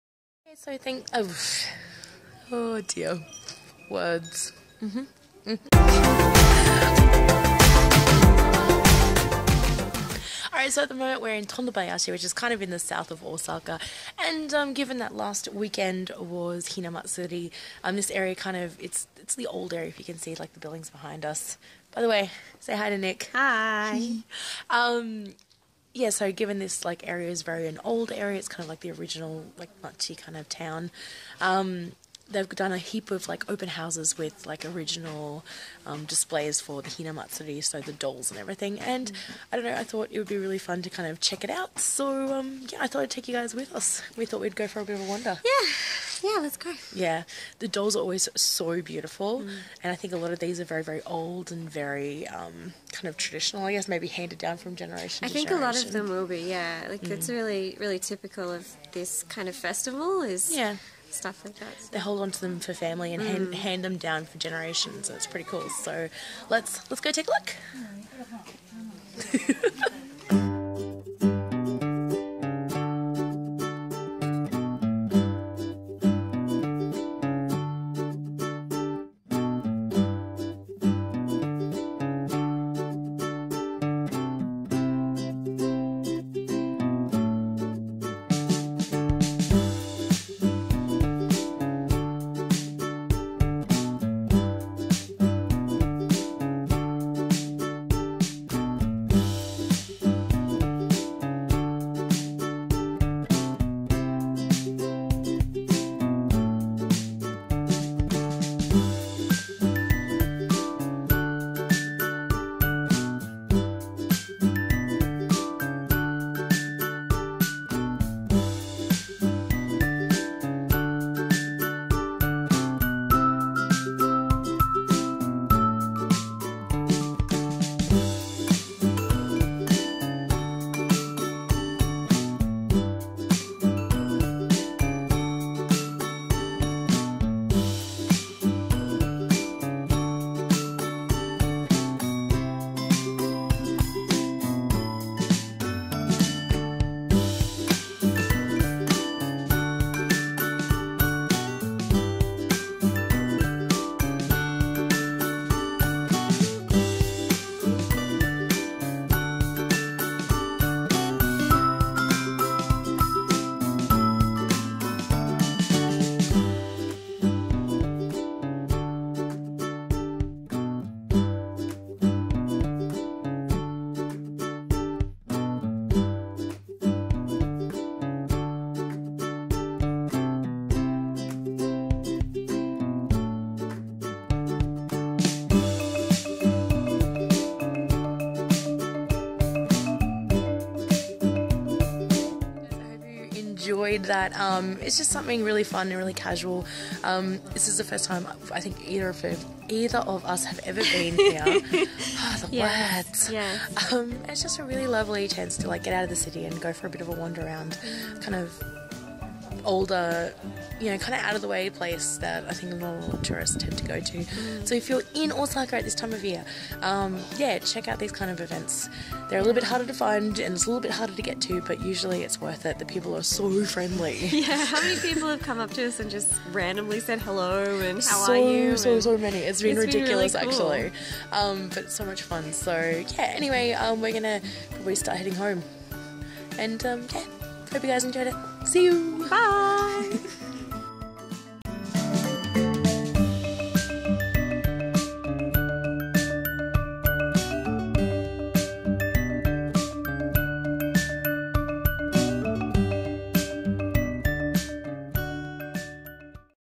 Hina Matsuri - Girl's Day Festival in Osaka
Every year in southern Osaka, families and businesses in this traditional neighbourhood open their homes and stores to display collections of Hina Matsuri or Girl's Day dolls to the public. Many of these dolls have been handed down for generations and give a great glimpse into the traditions and history of this festival in the Kansai region.